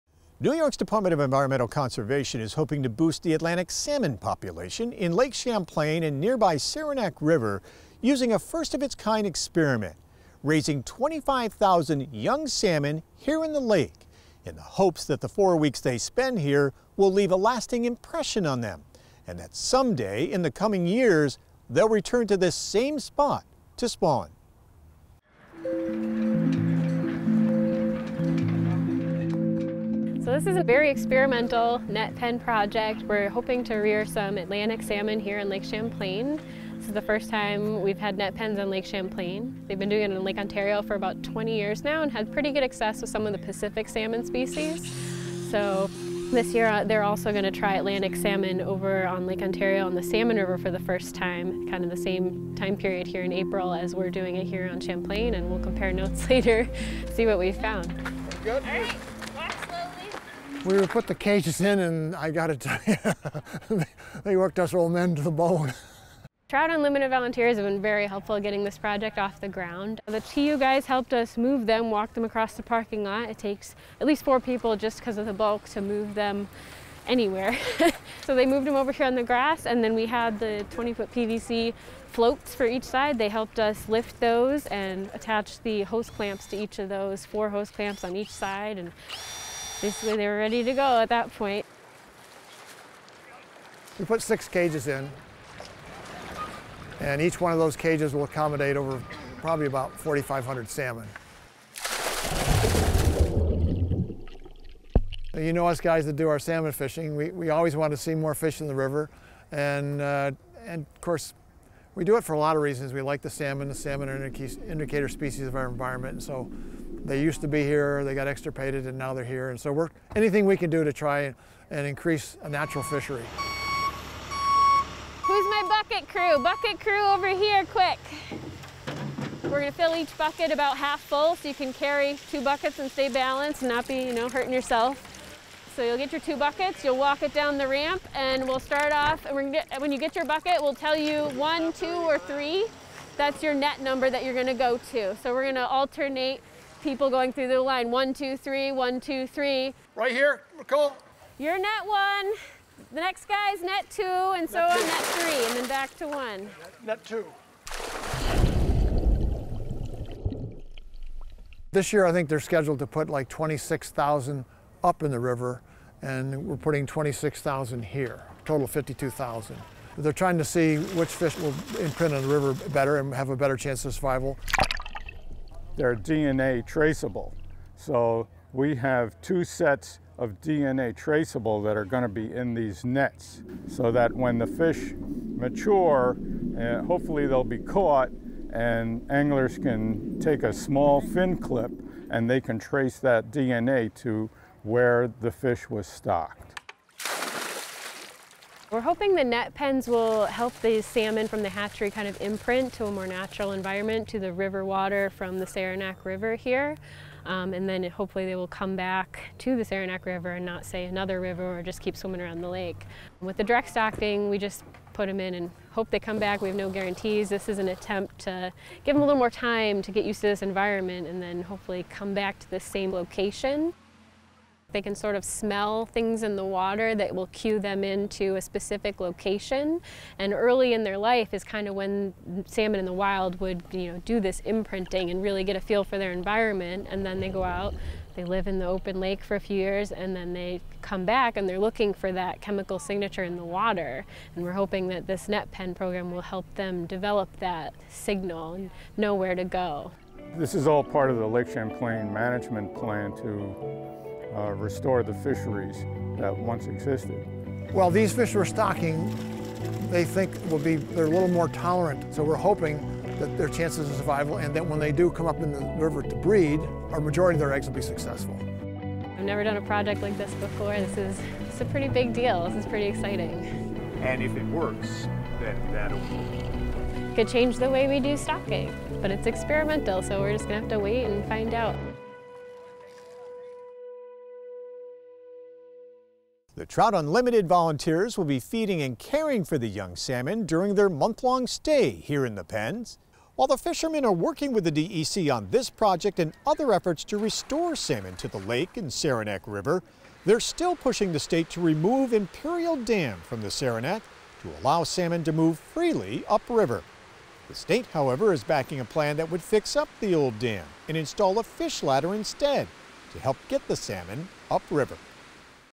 Atlantic Salmon Experiment on Lake Champlain
New York’s Department of Environmental Conservation is trying to boost the Atlantic Salmon population in Lake Champlain and Saranac River using a first-of-its-kind experiment to raise more than 25-thousand young salmon in the lake, in the hopes that the 4-weeks the fish spend there will leave a lasting impression on them and that someday, in a few years, they’re return to the same spot to spawn.